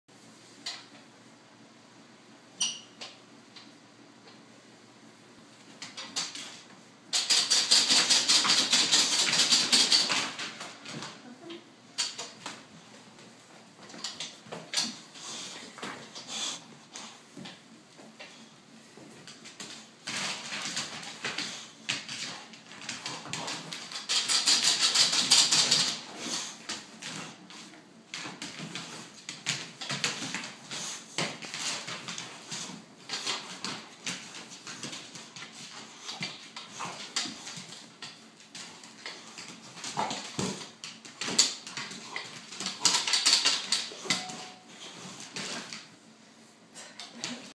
Boxer Puppy Pete Goes Crazy Over Dust Mop
We'd like to hone in his jumping skills, but this'll do for now. Pete is 8 months old, 45 pounds, and very excited about the dust mop.